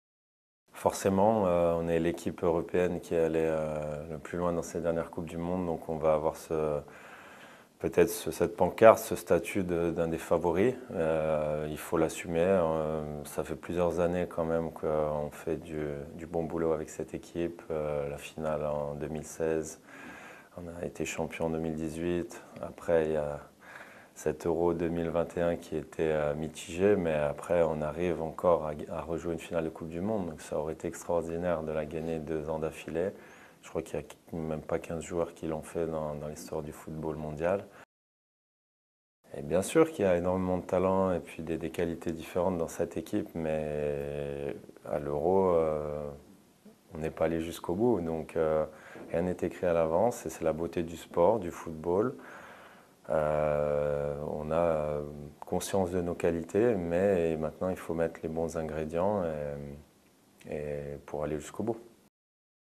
Giroud on France's strength & Les Bleus being favorite to win next European championship｜Euro 2024
France forward Olivier Giroud joked about his longevity saying he is like a good wine that gets better as time goes by.
The 36-year-old AC Milan striker was called by Les Bleus manager Didier Deschamps for the 2024 European Championships qualifiers against Gibraltar next Friday and against Greece next Monday.
France started their campaign with a resounding 4-0 win over the Netherlands in Paris, in a match where Kylian Mbappe scored a brace with Giroud coming in as a substitute for Randal Kolo Muani in the 77th minute.
Giroud was instead in the starting 11 when Deschamp's side claimed a 1-0 win in Dublin against Ireland with Benjamin Pavard scoring the winner.